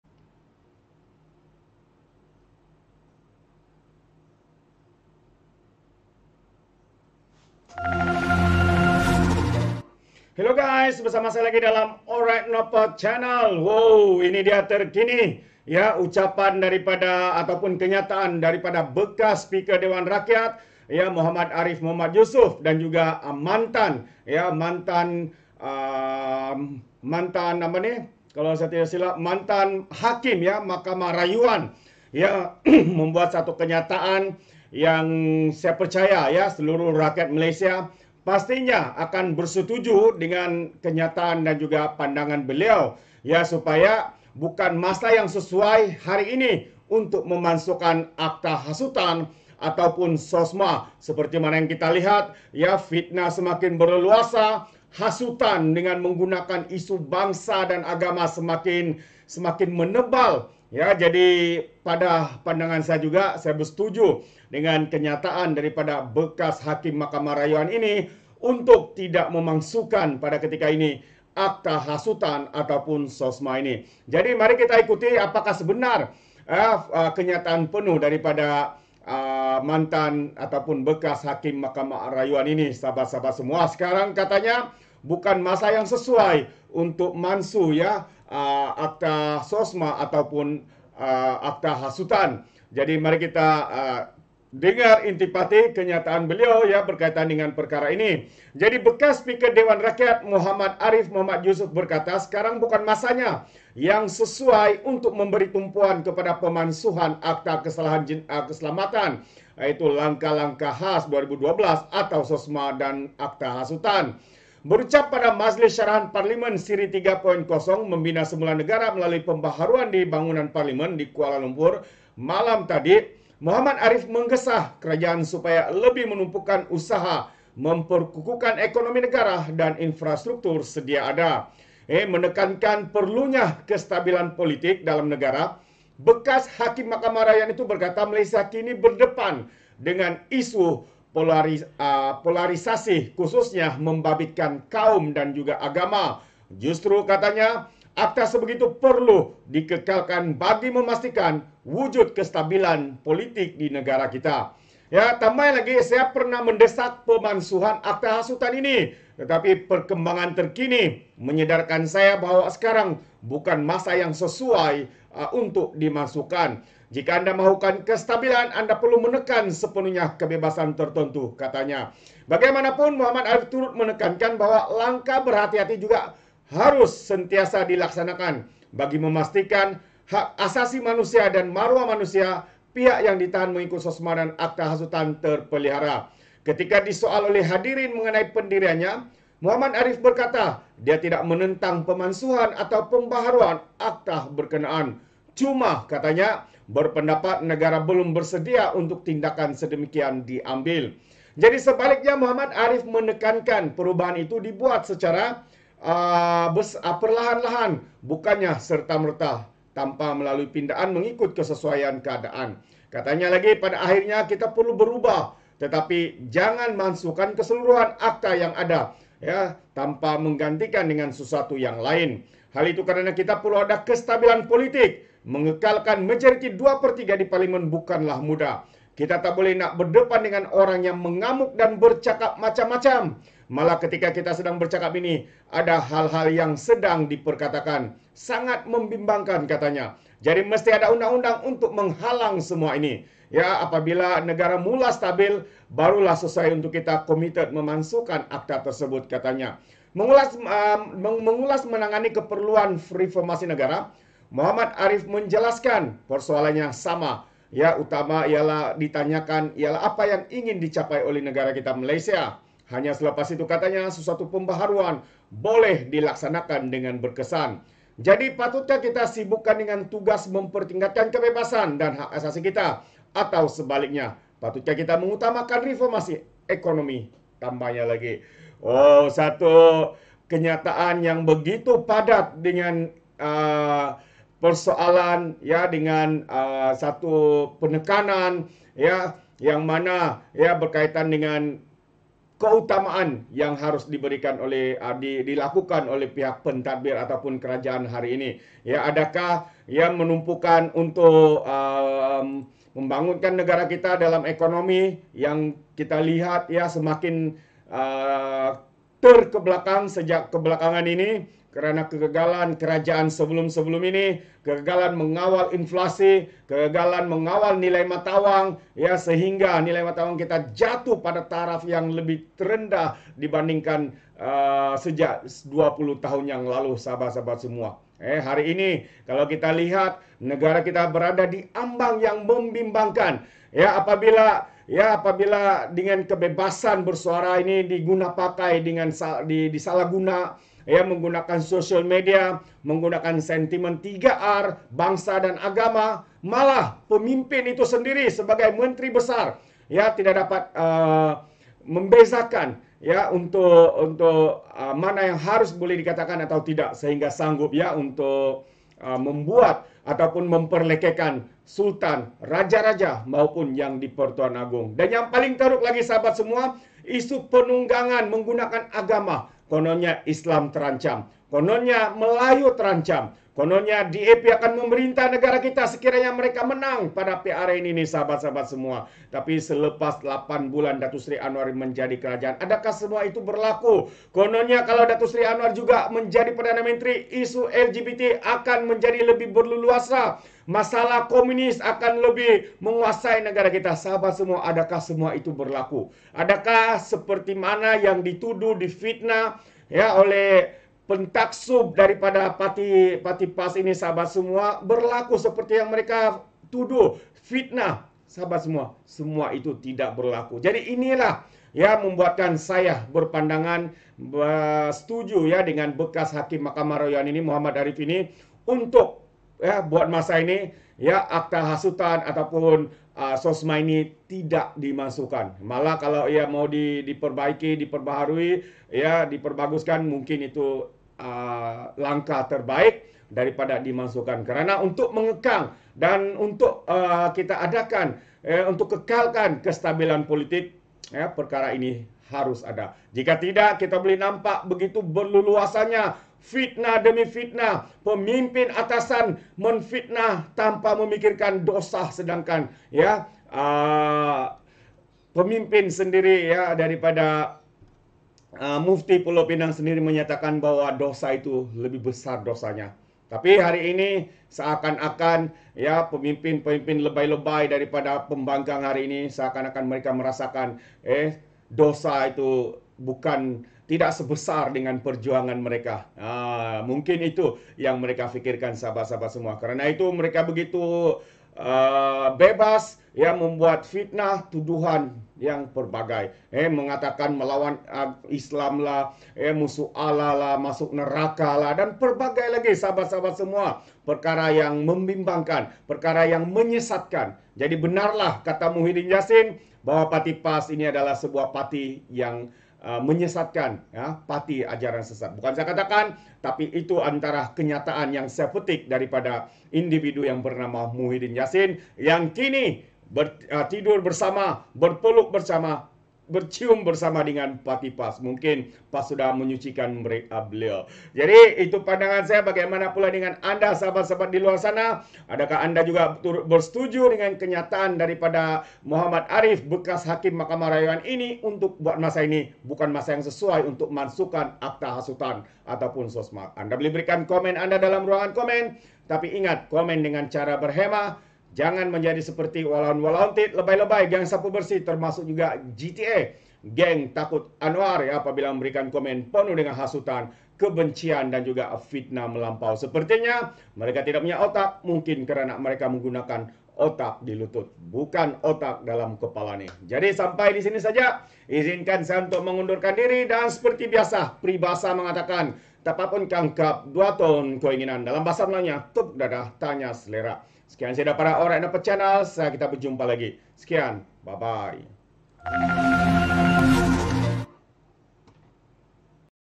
[terkini] BEKAS HAKIM MAHKAMAH RAYUAN!BUKAN MASA YANG SESUAI MANSUH AKTA HASUTAN!
Bekas Speaker Dewan Rakyat Mohamad Ariff Md Yusof berkata sekarang “bukan masa yang sesuai” untuk memberi tumpuan kepada pemansuhan Akta Kesalahan Keselamatan (Langkah-Langkah Khas) 2012 - atau Sosma - dan Akta Hasutan.
Berucap pada majlis Syarahan Parlimen Siri 3.0: Membina Semula Negara Melalui Pembaharuan di bangunan Parlimen di Kuala Lumpur malam tadi Mohamad Ariff menggesa kerajaan supaya lebih menumpukan usaha memperkukuh ekonomi negara dan infrastruktur sedia ada.
Menekankan perlunya kestabilan politik dalam negara, bekas Hakim Mahkamah Rayuan itu berkata Malaysia kini berdepan dengan isu polarisasi khususnya membabitkankaum dan agama.